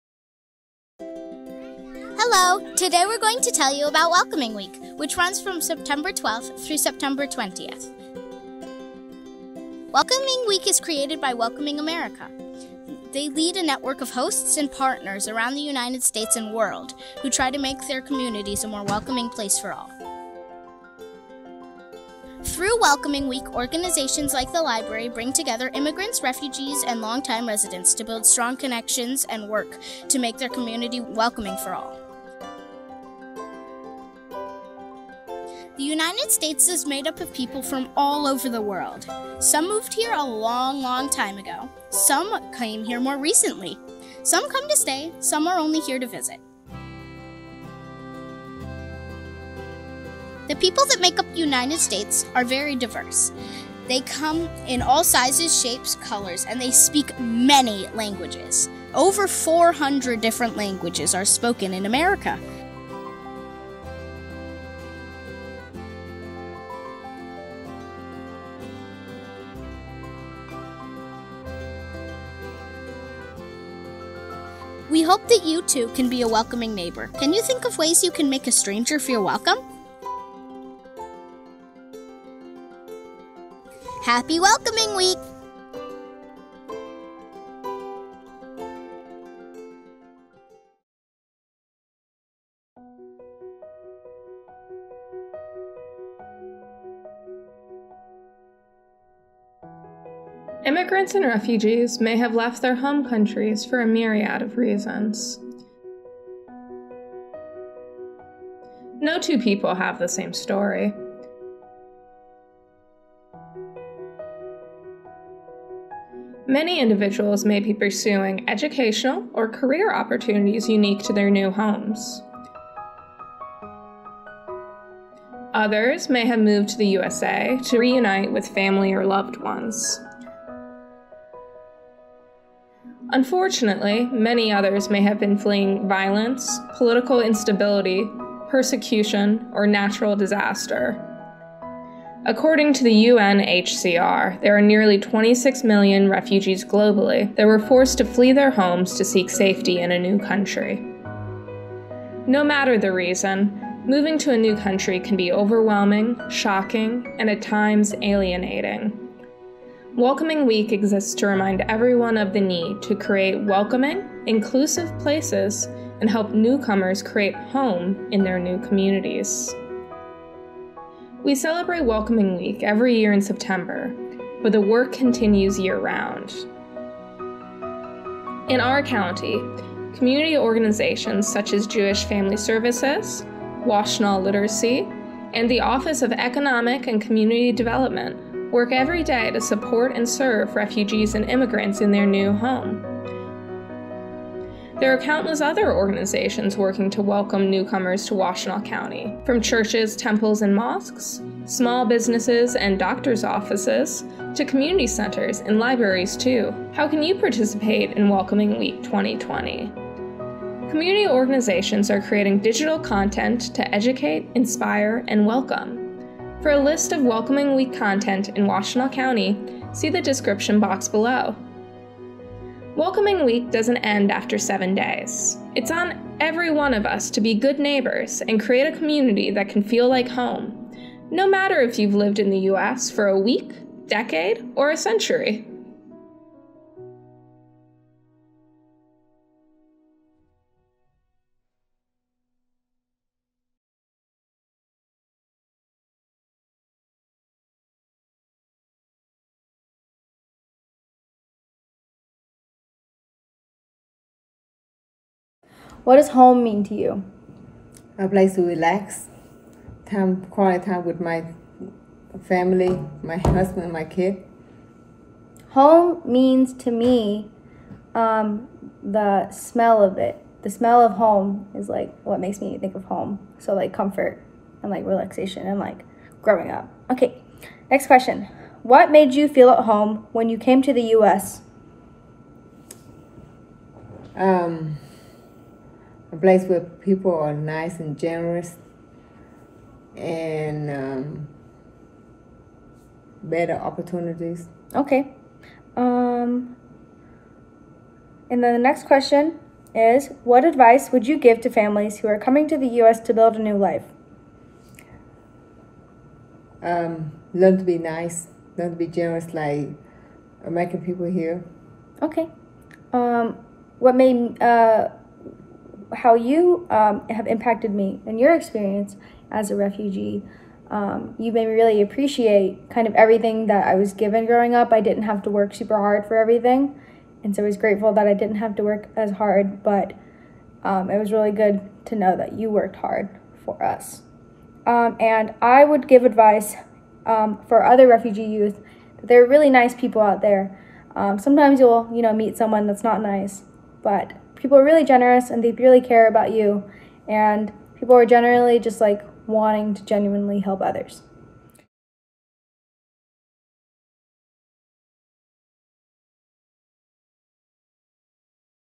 What is Welcoming Week?
Join the Office of Economic Development of Washtenaw County, Jewish Family Services and the Michigan Immigrant Rights Coalition and learn how to be a welcoming community member!

Welcoming America is proud to lead the growing network of hosts and partners around the United States and world who strive to make their communities a more welcoming place for all.

Through Welcoming Week, organizations and communities bring together immigrants, refugees, and long-time residents to build strong connections and affirm the importance of welcoming and inclusive places in achieving collective prosperity.